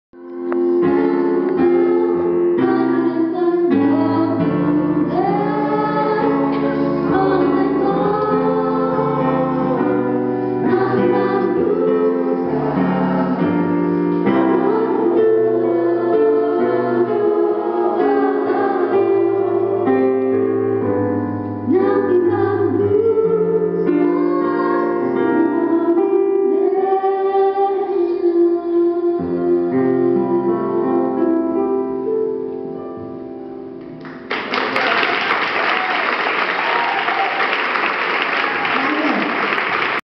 Gospel kontzertua (Hernani, 2014-06-06)
Ainara Ortega hernaniarrak Donostian martxan jarritako Inalambrika Musika eta Antzerki Eskolako korua, gospel kontzertua ematen Hernanin.